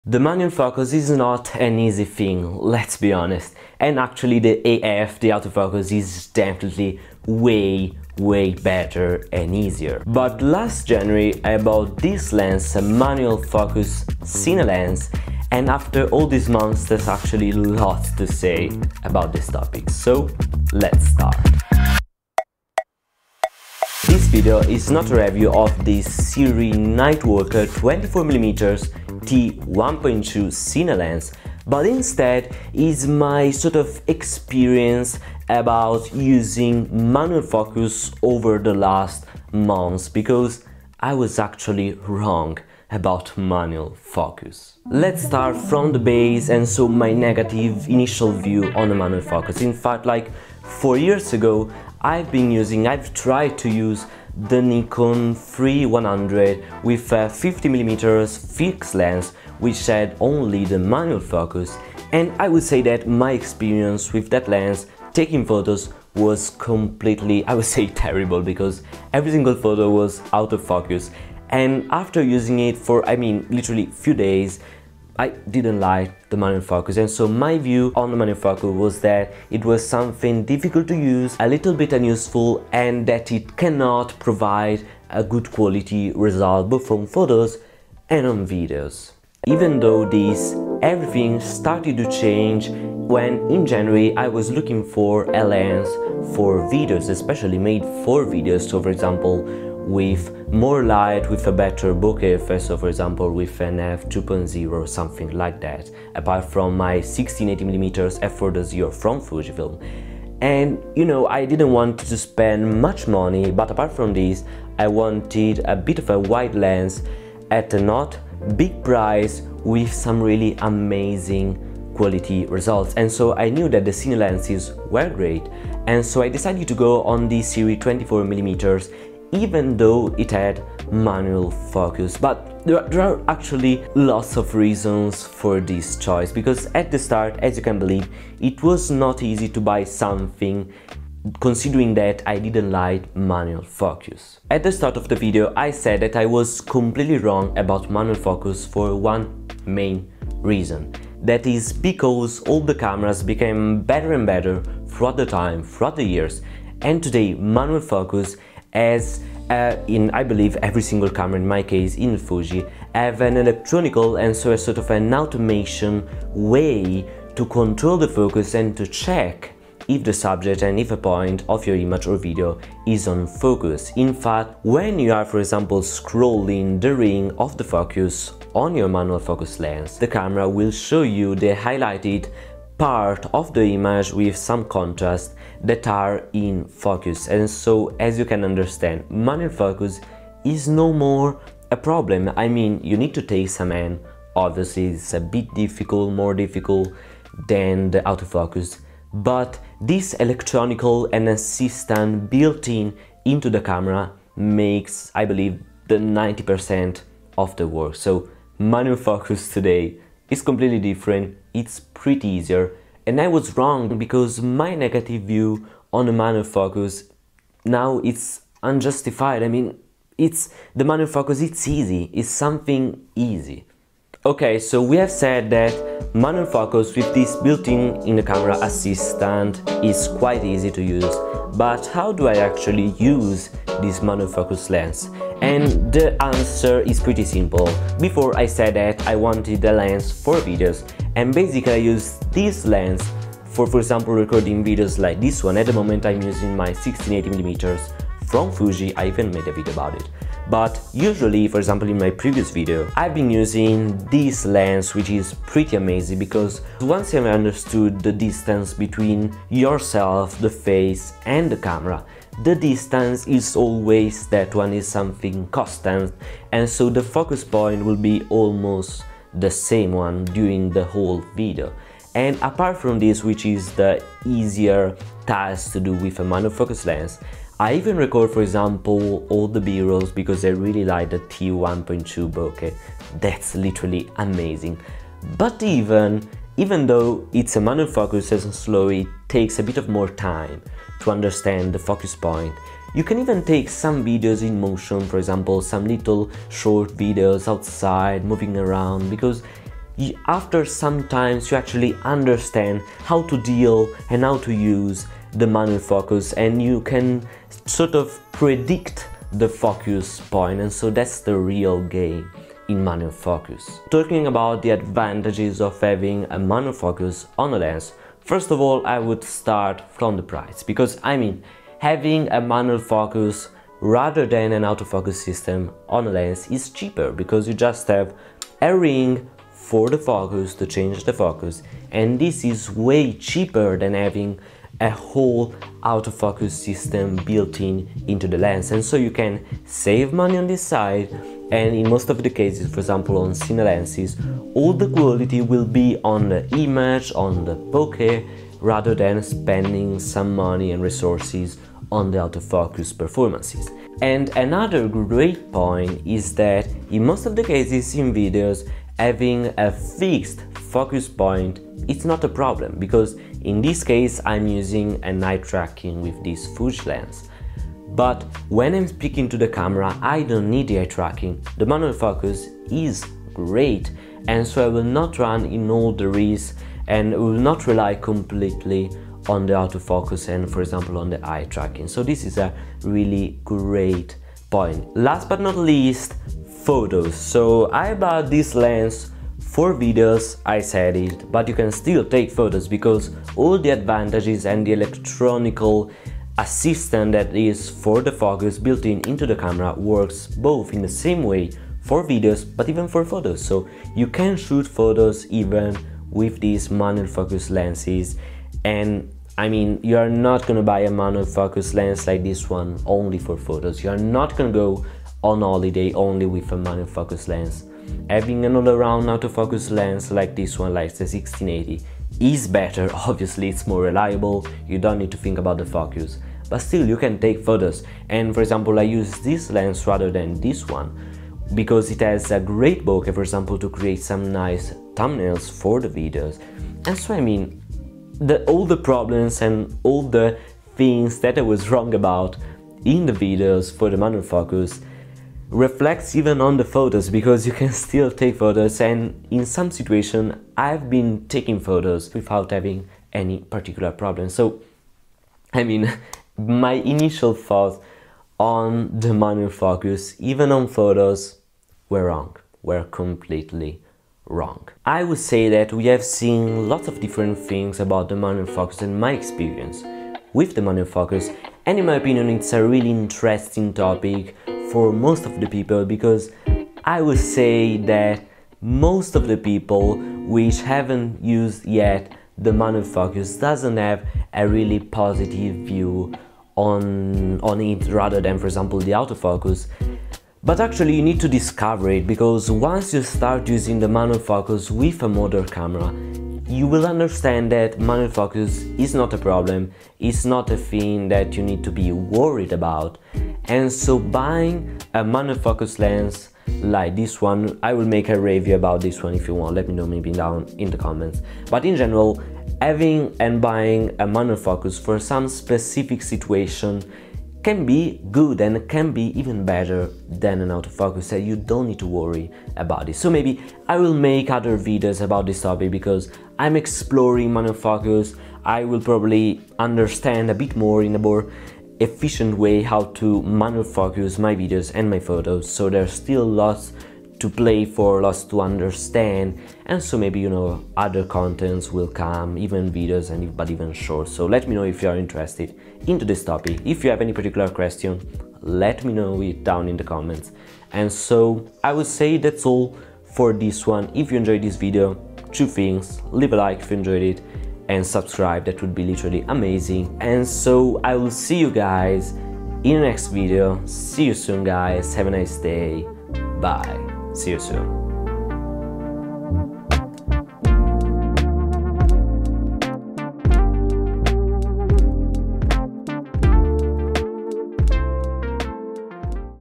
I was WRONG about Manual Focus
Probably you hate Manual Focus, especially if you take mainly photos, but in this video I’d like to go through my first months with a full Manual Focus Cine Lens.
My view has quite changed on MF, so there’s lot to say about it, check out the entire video to find out more!

Chapters:
0:00 intro
0:44 my initial view
1:29 the change
2:24 why I was wrong
3:44 Manual Focus in Videos
6:24 Manual Focus in Photos
7:45 It's not that bad
9:11 outro